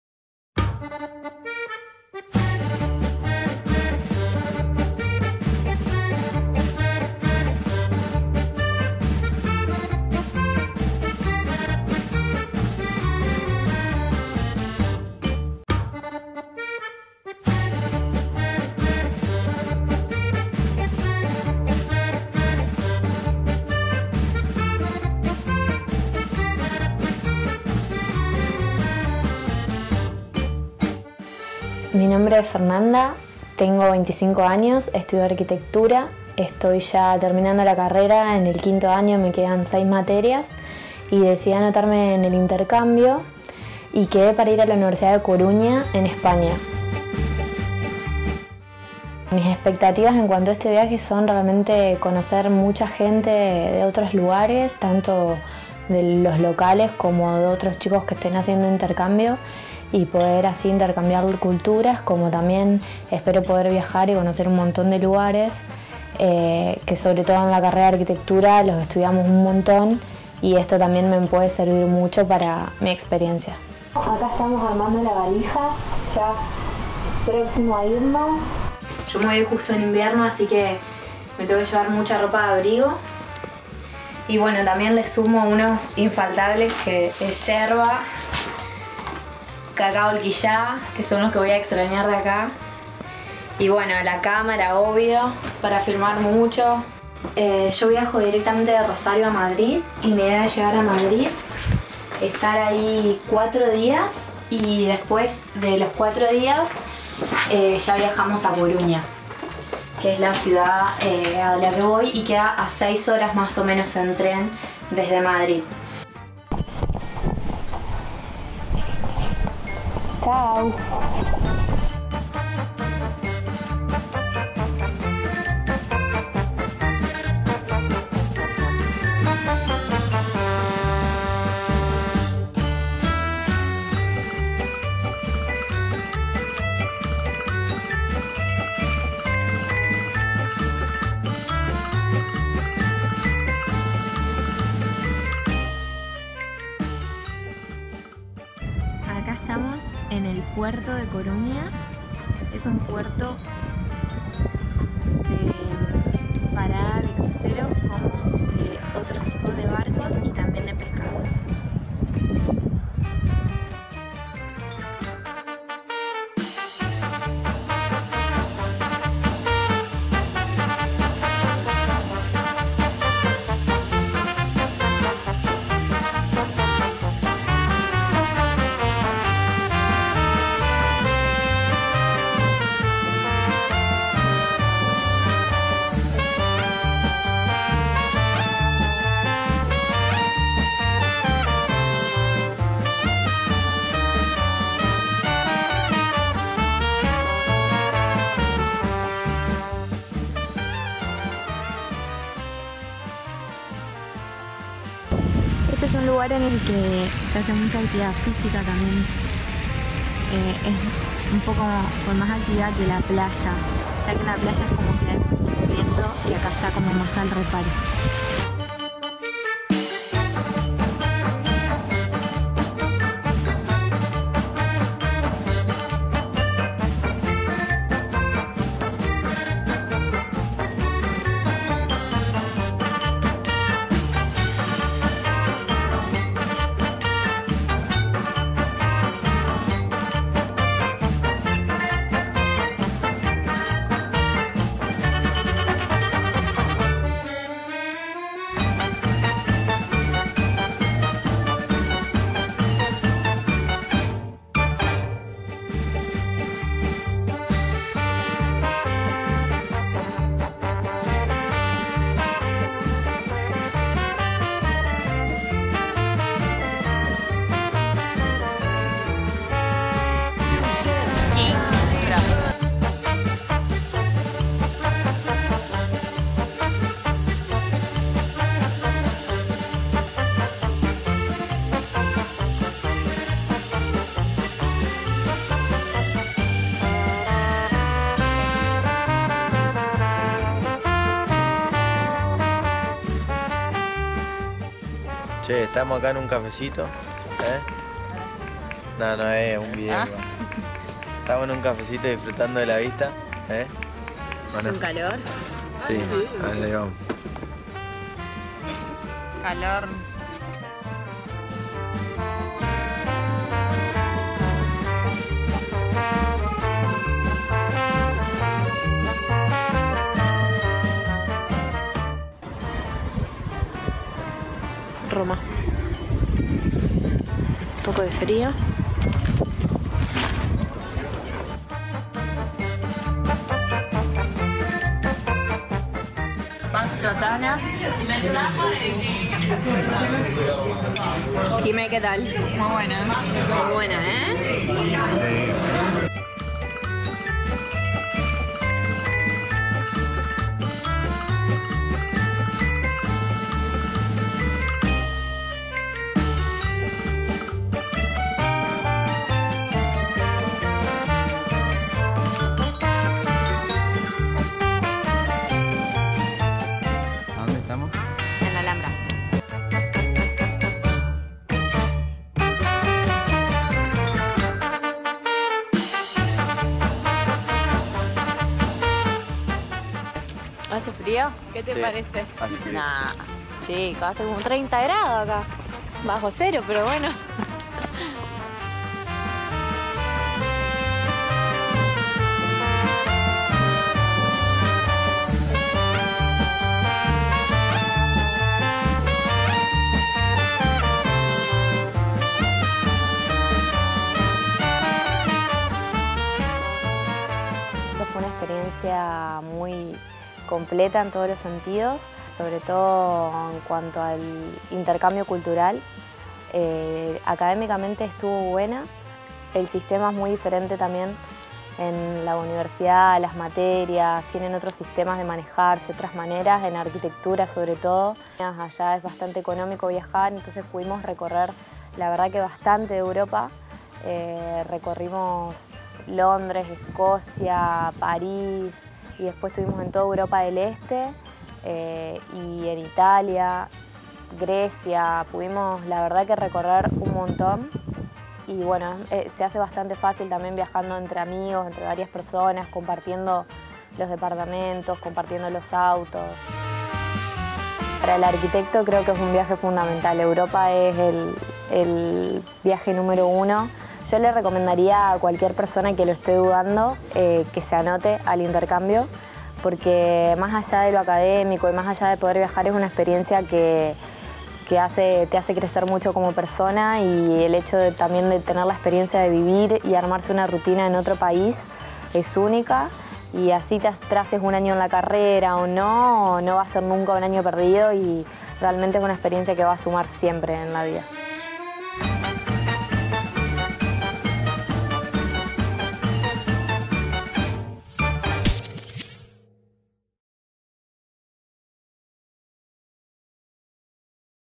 Por el Mundo - Fernanda/España
Fernanda Nessier es argentina, estudiante de Arquitectura en la Facultad de Arquitectura y Diseño Urbano (FADU) y realizó un intercambio a la Universidad de la Coruña, en La Coruña, España.